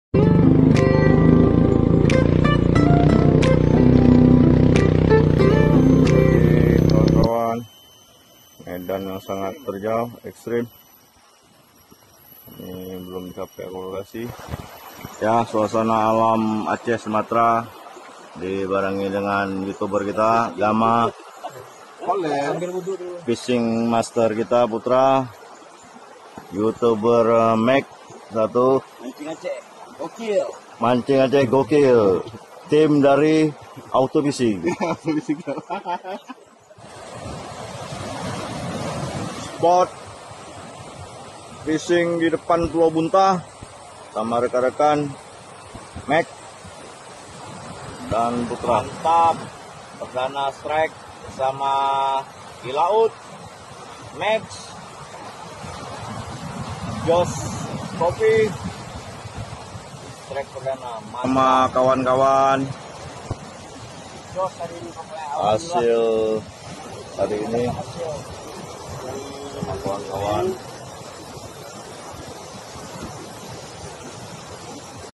Mancing Asyik Lhok Ketapang Cut ll Strike Blue Fin ll Aceh Indonesia
Editor : Capcut

Collab :
Mancing Aceh Gokil